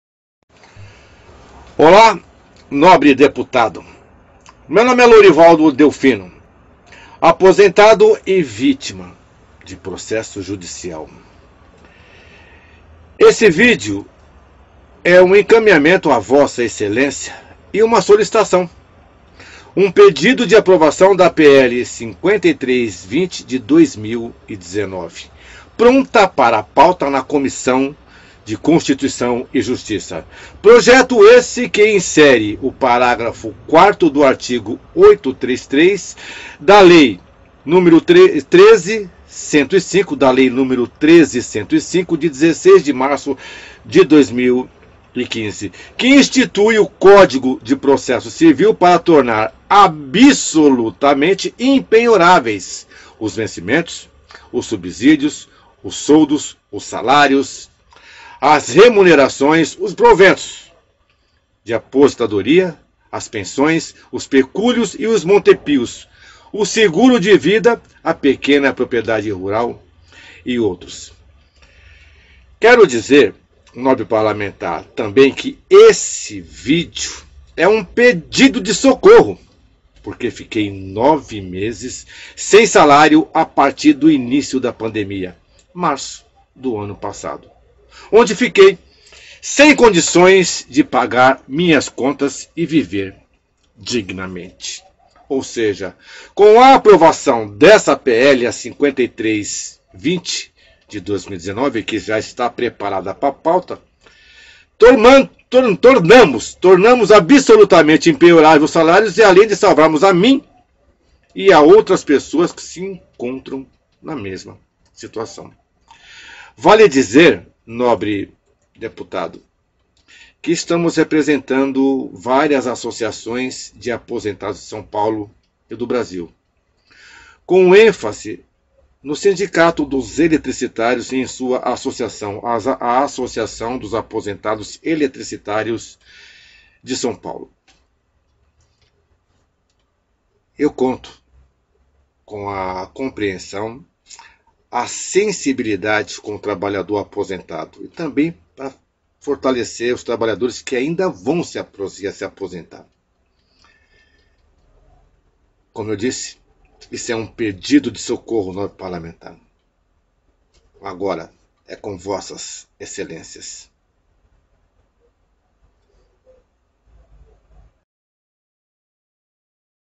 VPR CF 21 02070298 Aprovação da PL 5320 2019 pedido de socorro ao Dep Lucas Redecker
PL 5320/2019
                                                 Situação:  CCJ - Pronta para Pauta
Ementa:  Insere §4º ao Art. 833 da Lei nº 13.105, de 16 de março de 2015, que institui o Código de Processo Civil, para tornar absolutamente impenhoráveis os vencimentos, os subsídios, os soldos, os salários, as remunerações, os proventos de aposentadoria, as pensões, os pecúlios e os montepios, o seguro de vida, a pequena propriedade rural e outros.
Data último despacho:  03/10/2019 17:11:00